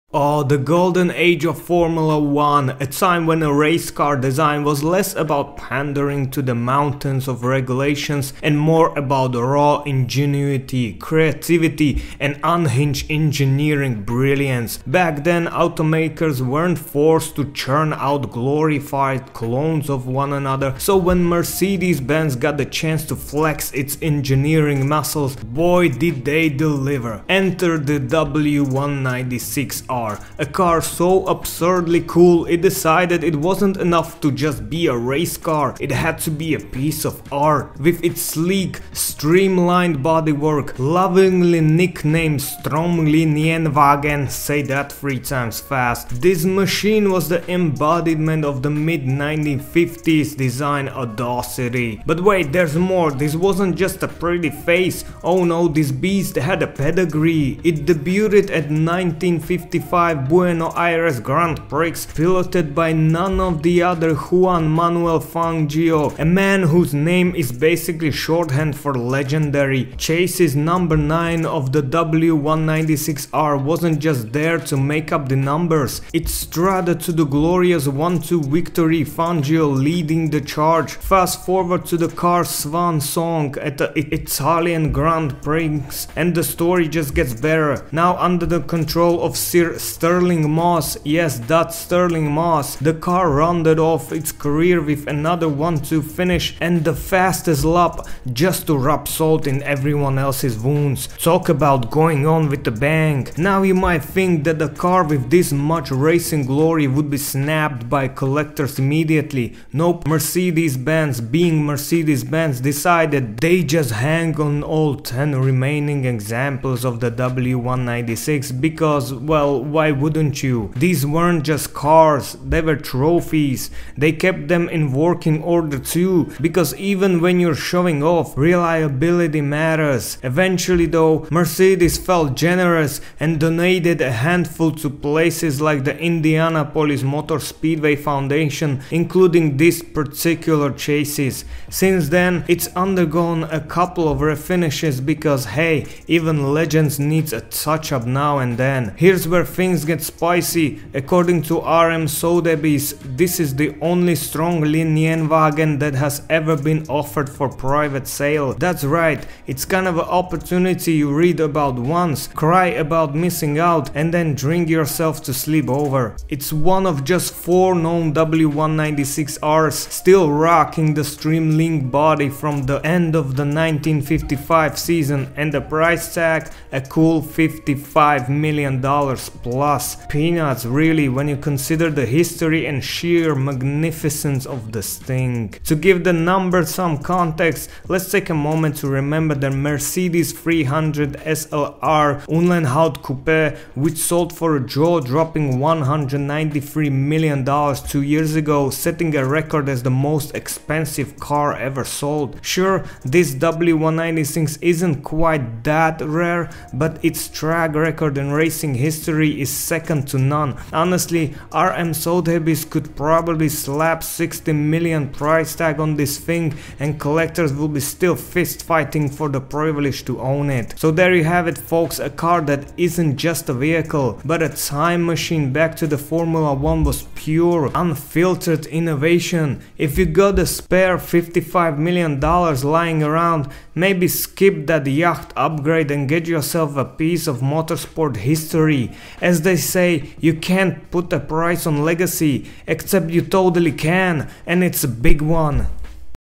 Mercedes Benz W 196 R The $55 Million F1 Icon with a Legendary Racing History!
Dive into the incredible story of the Mercedes-Benz W 196 R Stromlinienwagen, a Formula One masterpiece from the 1950s. With legendary drivers like Juan Manuel Fangio and Stirling Moss at the wheel, this streamlined beauty dominated the racing world, securing unforgettable victories at the Buenos Aires Grand Prix and Italian Grand Prix.
Now, this rare piece of motorsport history is hitting the auction block at RM Sotheby’s, with an estimated value of $55 million! Discover why this car is not just a vehicle but a priceless relic of racing innovation.
🚗 Key Highlights:
• Racing debut: 1955 Buenos Aires Grand Prix
• Legendary drivers: Juan Manuel Fangio & Stirling Moss
• Final race: 1955 Italian Grand Prix (1-2 finish & fastest lap)
• Streamlined bodywork: Only 4 known examples exist!
• Auction estimate: Over $55 million!
Don’t miss this deep dive into one of the most iconic cars in Formula One history. If you love vintage cars, motorsport heritage, or jaw-dropping auction stories, this is the video for you!